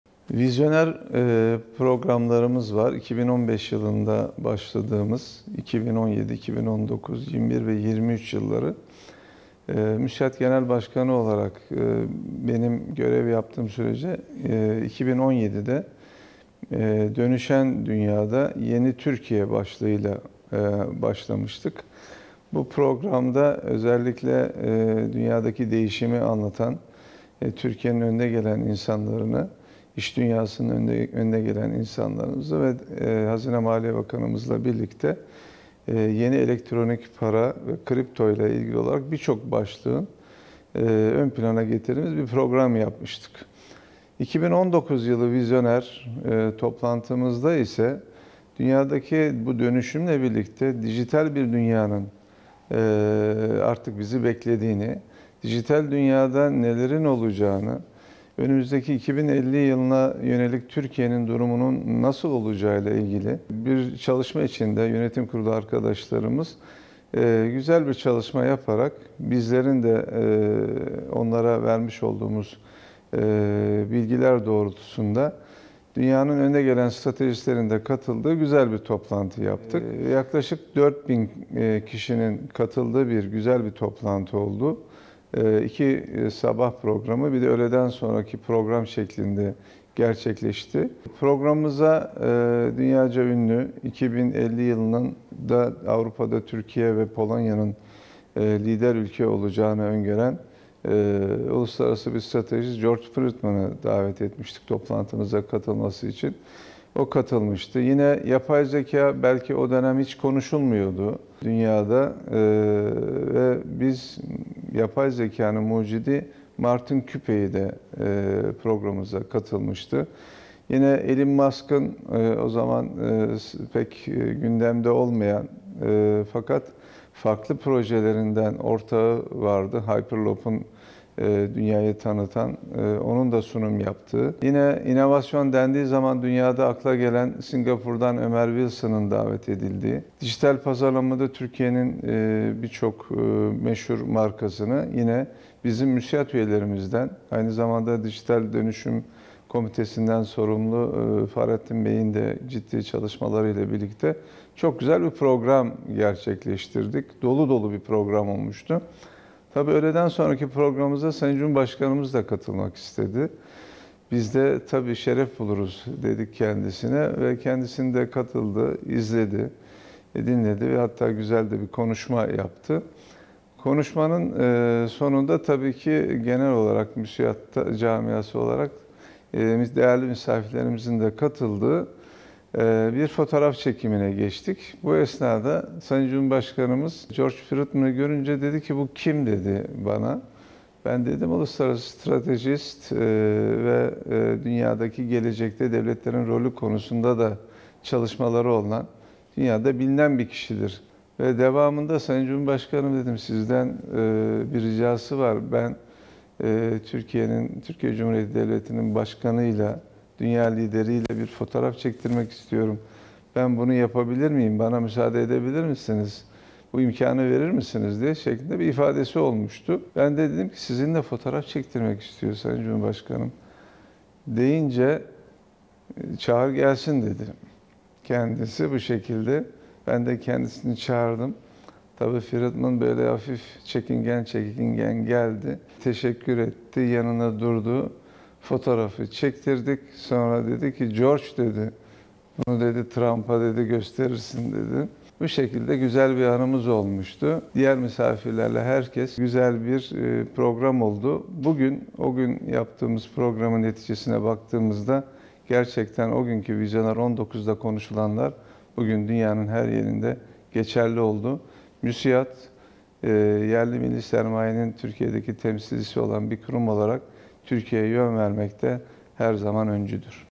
Vizyoner 2019'da Neler Yaptık | Abdurrahman KAAN
MÜSİAD altıncı Dönem Genel Başkanı Abdurrahman KAAN, MÜSİAD'ın 34'üncü yılına özel yapılan
"Konuşan Fotoğraflar Sergisi" programında, kendi dönemindeki önemli fotoğrafların hikâyelerini anlattı.